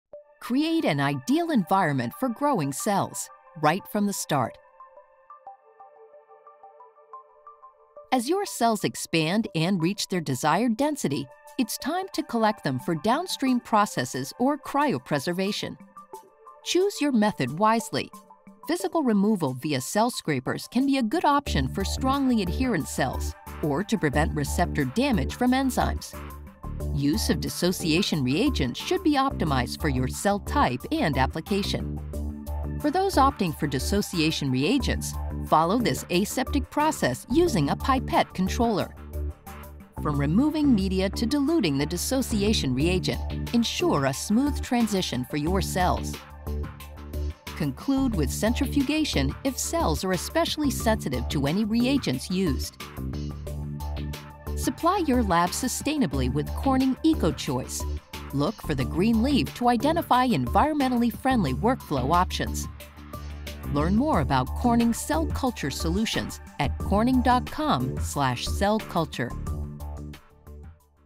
How to Create an Ideal Environment for Growing Cells: Harvest Phase
In the dynamic world of cell culture, precision and efficiency are paramount at every phase of your workflow. Our comprehensive solutions help you create an ideal environment for growing cells, right from the start.

Watch this third video which explains how Corning solutions support your Harvest stage of Cell Culture Workflow. Click the links below to check our products.